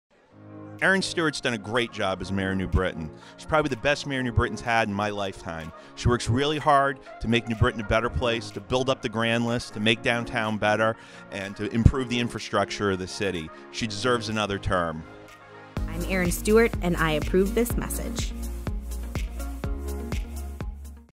Richard Wareing Supports Mayor Erin Stewart: Testimonial Tuesday
“She’s probably the best Mayor New Britain has had in my lifetime.”

In this TestimonialTuesday video, Richard Wareing talks about why he is supporting Mayor Erin Stewart this November.